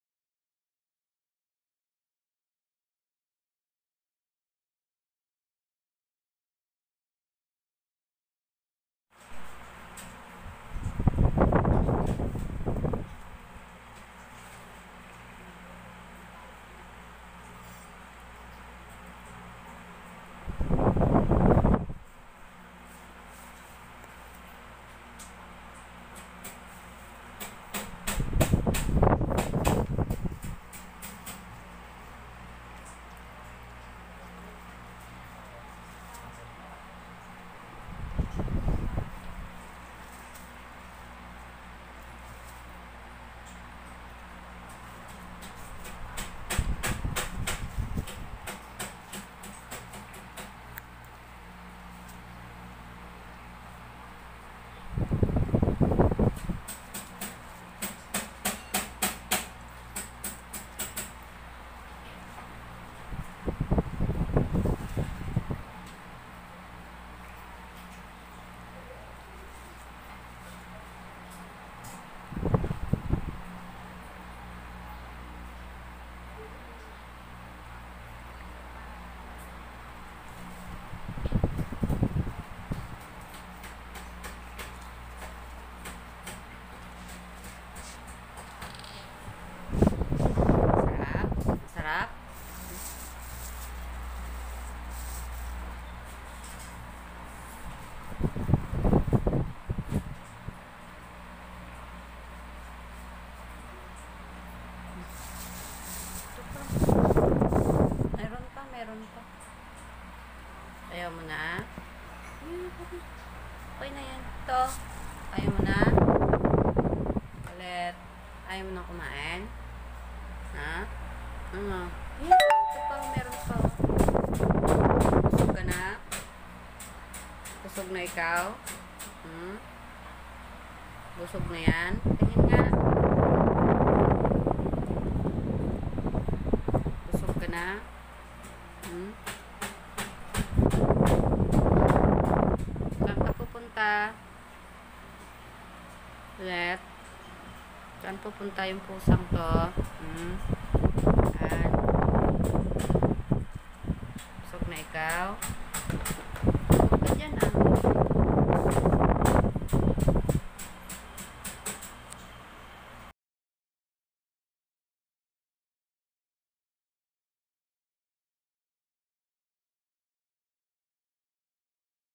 Cat Favorite food sweetcorn
my cat loves to eat sweetcorn